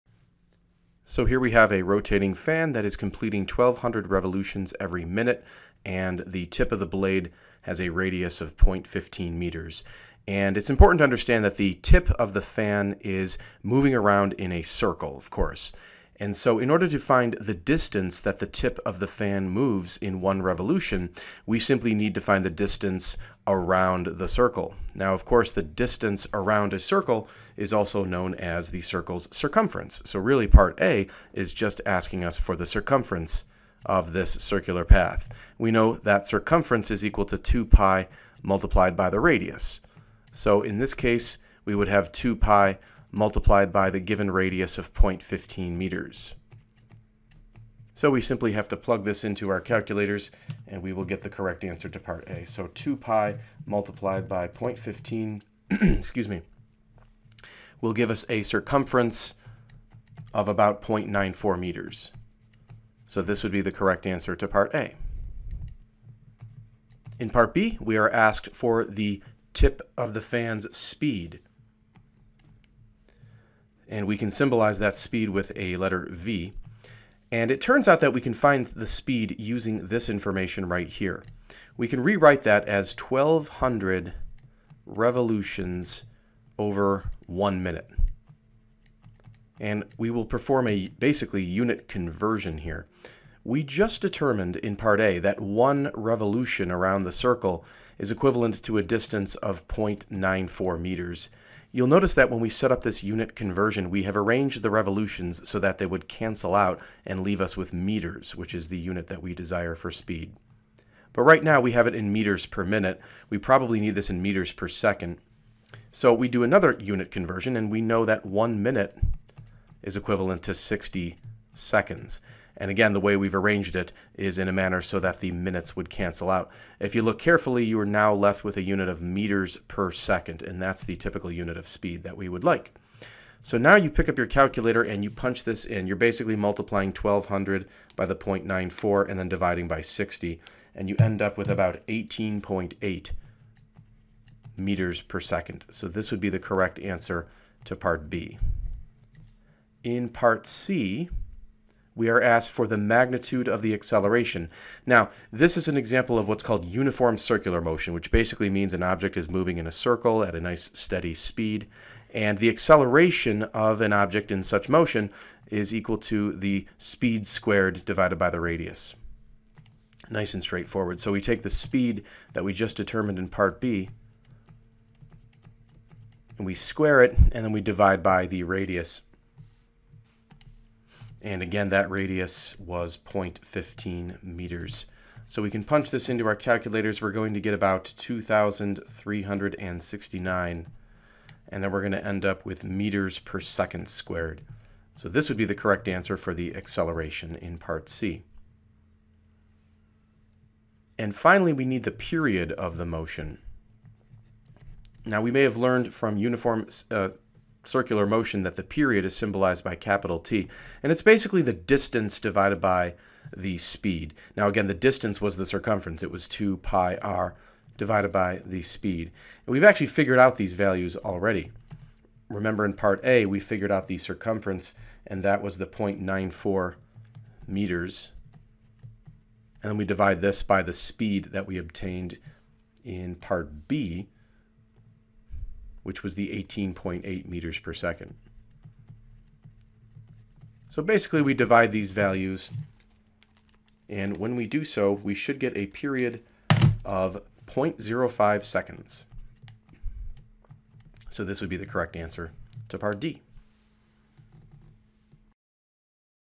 A rotating fan completes 1200 revolutions
A rotating fan completes 1200 revolutions every minute. Consider the tip of a blade, at a radius of 0.15 m. (a) Through what distance does the tip move in one revolution? What are (b) the tip's speed and (c) the magnitude of its acceleration? (d) What is the period of the motion?